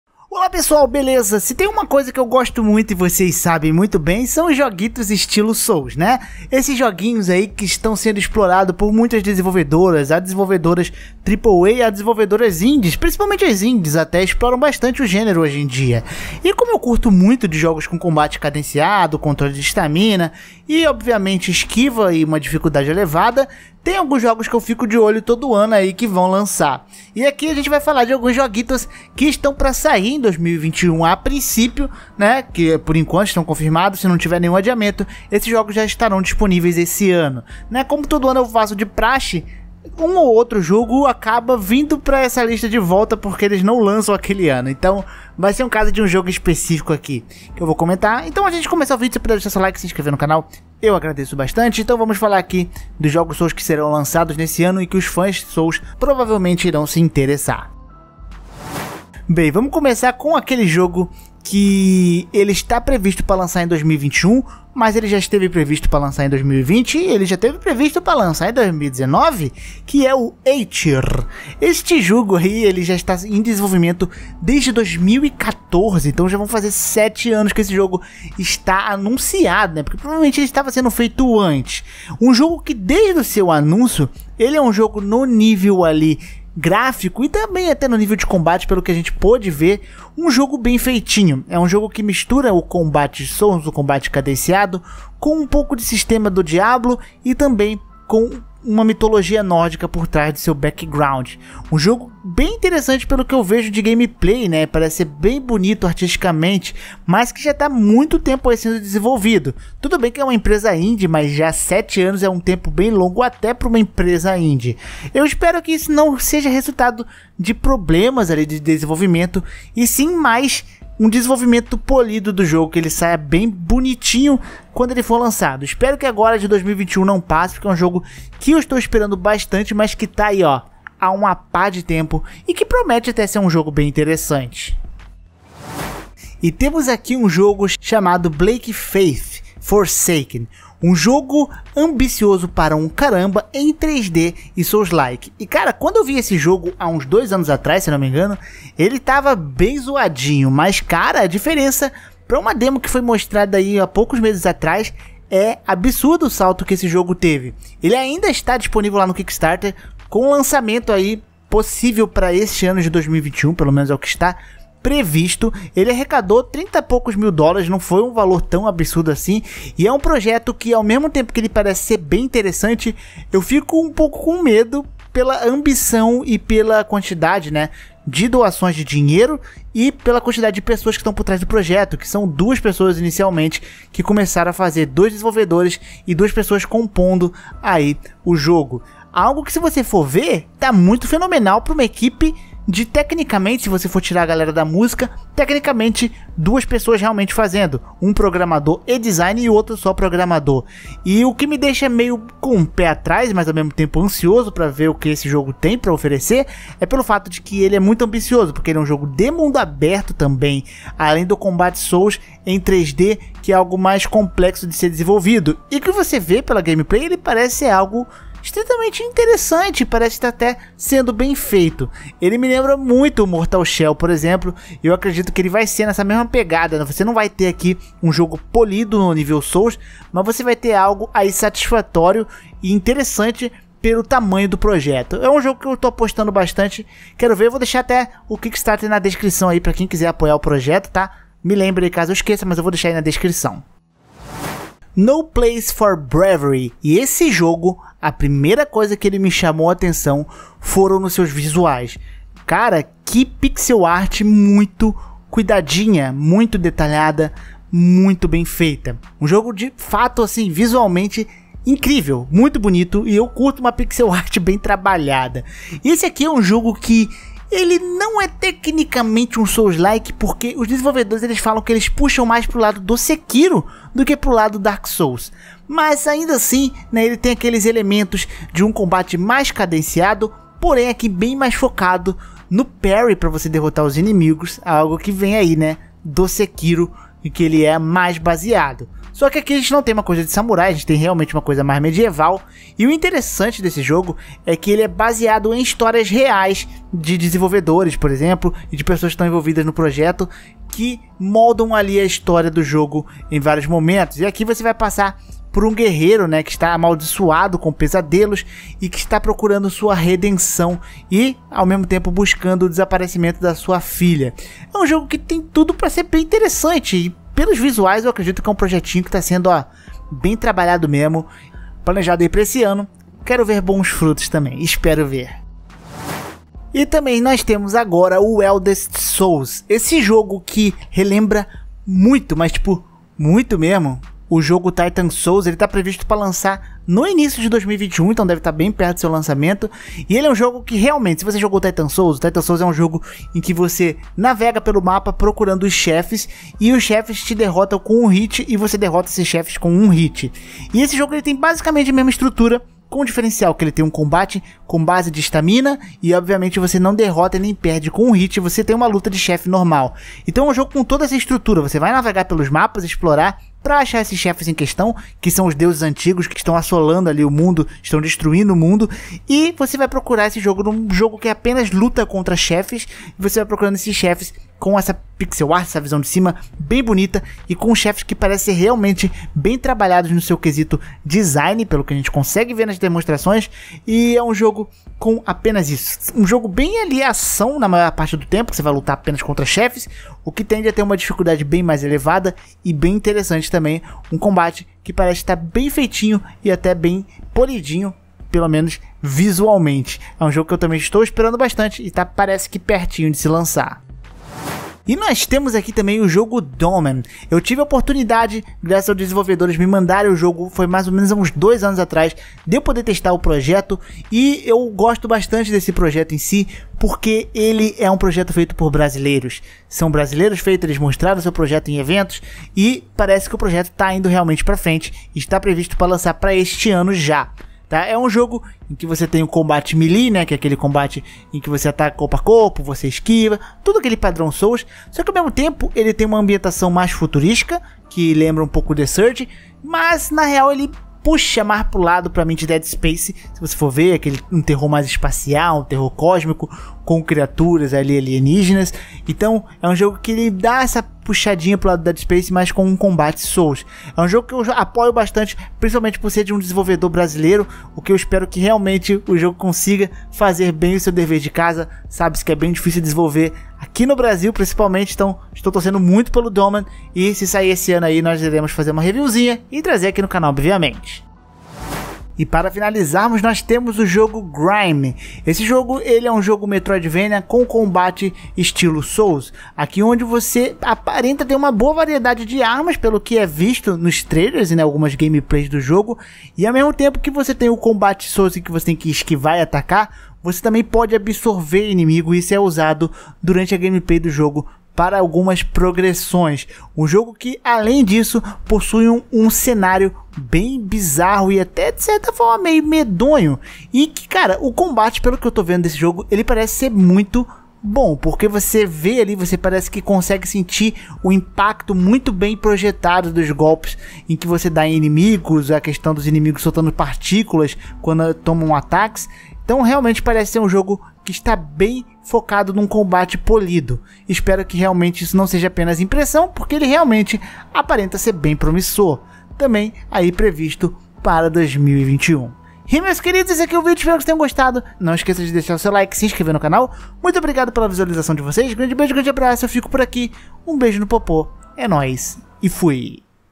Jogos que os fãs de DARK SOULS vão querer jogar em 2021!
Vamos ver os jogos estilos souls / souls like de 2021!

➤ PSN: hadessama94